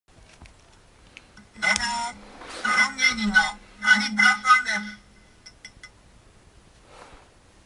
喋るドローン！！ Talking drone!! KISS ESC32A
喋るドローンだよ！

Talking drone!! taking in Japanese.
"Hi I am a drone Performer,Taniplus1"